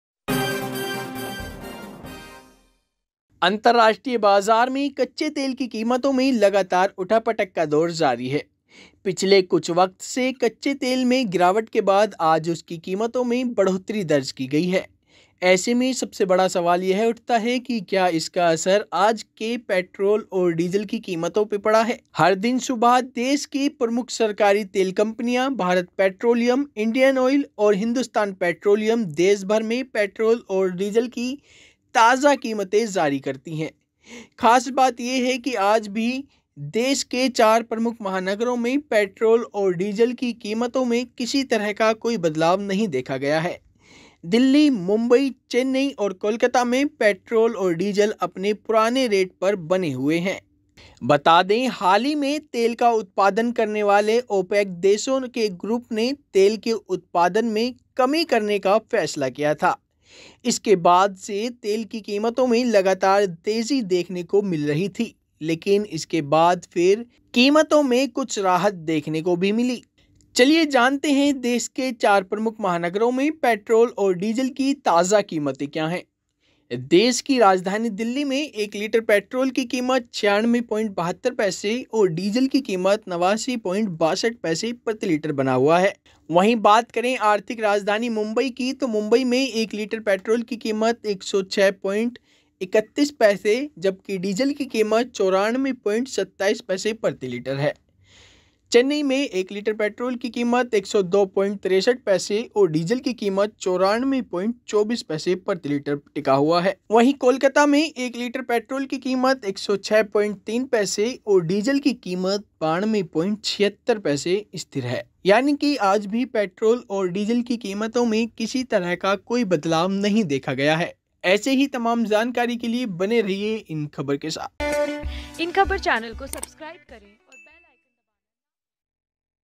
Petrol-Diesel Price: पेट्रोल-डीजल कंपन‍ियों ने जारी की ताजा कीमत,जानें अपने शहर के लेटेस्‍ट रेट
अंतरराष्ट्रीय मार्केट में कच्चे तेल की कीमतों (Crude Oil Price) में लगातार उथल-पुथल का दौर जारी है. पिछले कुछ वक्त से कच्चे तेल में गिरावट के बाद आज उसकी प्राइस में बढ़ोतरी दर्ज की गई है. ऐसे में सबसे बड़ा सवाल यह उठाता है